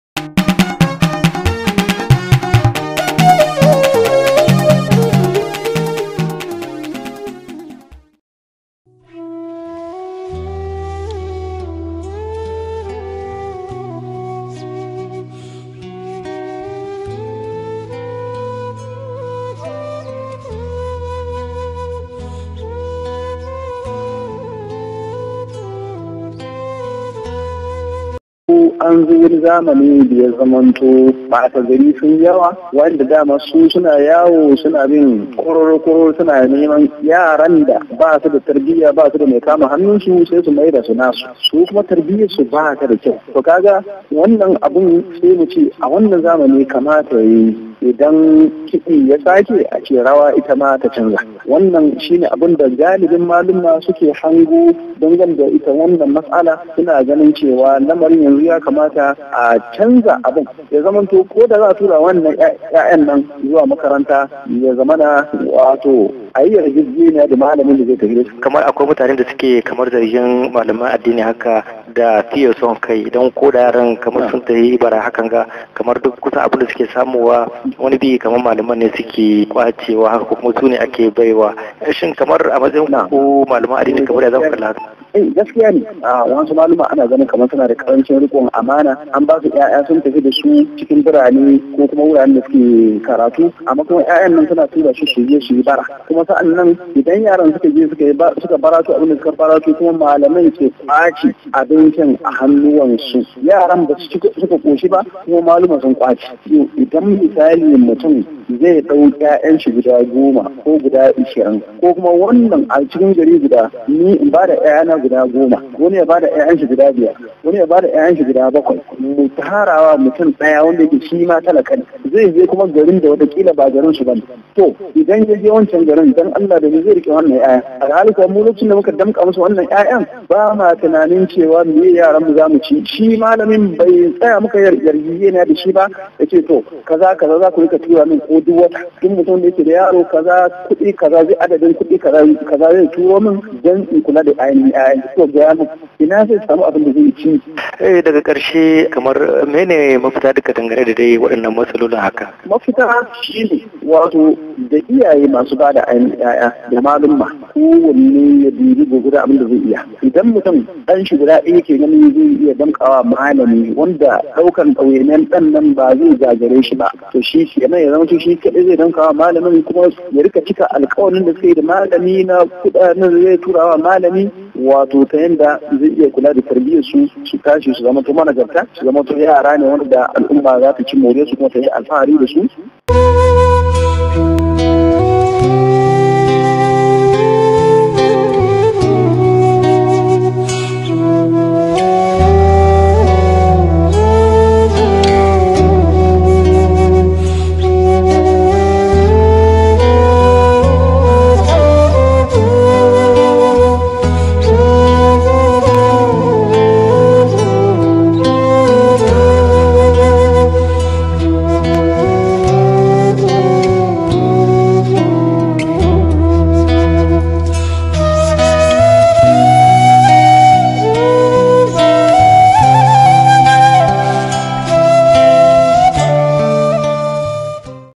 Bayani Akan Almajirci da bara daga bakin Shehu Sulaiman Agadez
Sairul Qalbi TV ke gabatar muku wannan shiri